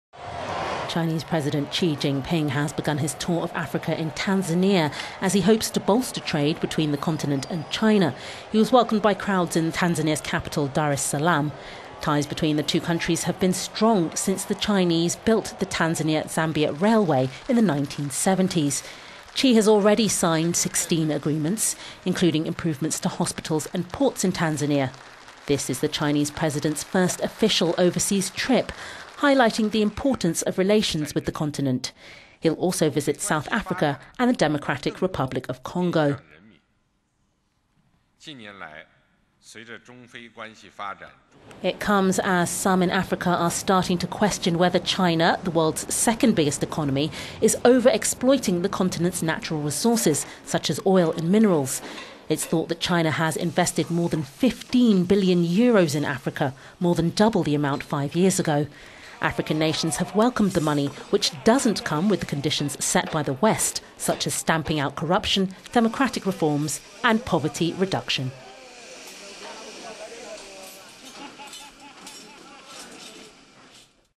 China's new president tours Africa to boost trade and investment
Chinese president Xi Jinping has begun his tour of Africa in Tanzania, as he hopes to bolster trade relations between the continent and China.

Ties between the two countries have been strong since the Chinese built the Tanzania-Zambia Railway in the 1970s.

Xi has already signed 16 agreements, including improvements to Tanzania's hospitals and ports..

Xi took office on 14 March and this is first official overseas trip, highlighting the importance of relations with the continent. He will also visit South Africa and the Democratic Republic of Congo.

The tour comes as some in Africa are starting to question whether China, the world's second biggest economy, is over-exploiting the continent's natural resources such as oil and minerals.

Trade between China and Africa is worth more than 150 billion euros. It is thought that China has invested more than 15 billion euros in Africa, more than double the 2007 total.

African nations have welcomed the money, which does not come with the conditions set by the West, such as stamping out corruption, democratic reforms and poverty reduction.